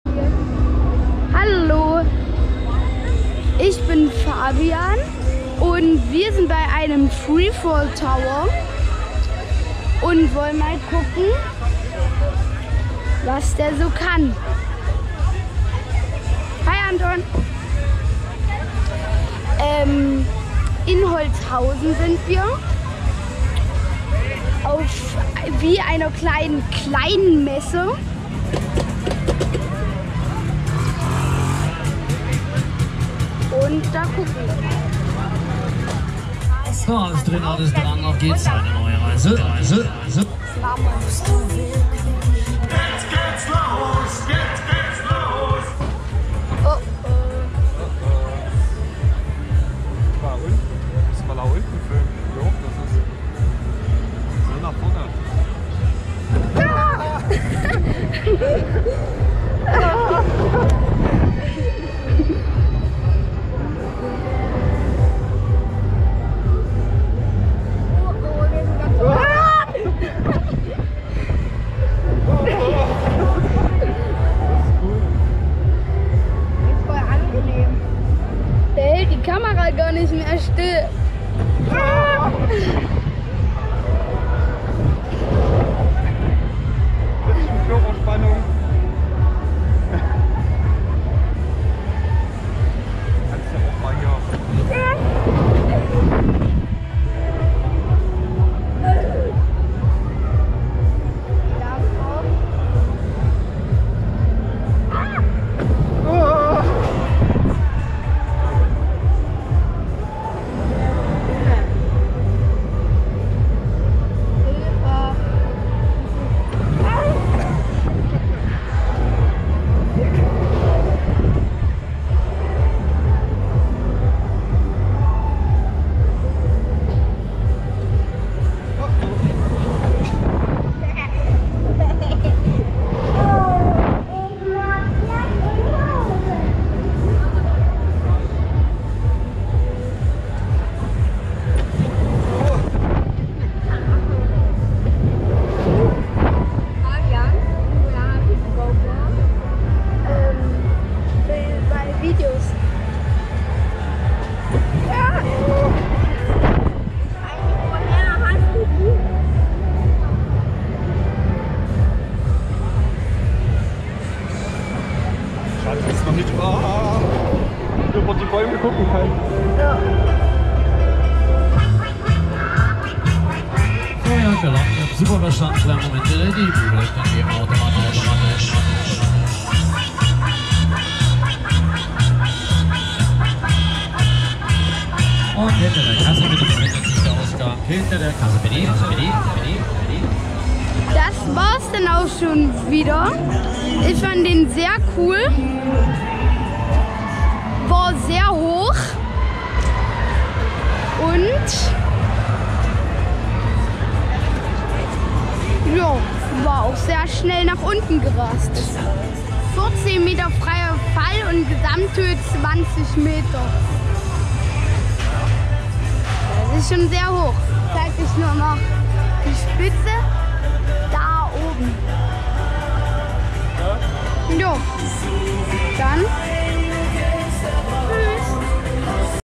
Free Fall (Nachtigall) - Onride - Volksfest Holzhausen 2021
Free Fall (Nachtigall) ist eine Free Fall Tower von SBF-Visa. Aufgenommen auf dem Volksfest Holzhausen 2021.

Der Drop Tower ist mit seinen 20m nicht der größte Tower, aber macht trotzdem ganz viel Spaß und man hat eine coole Airtime :D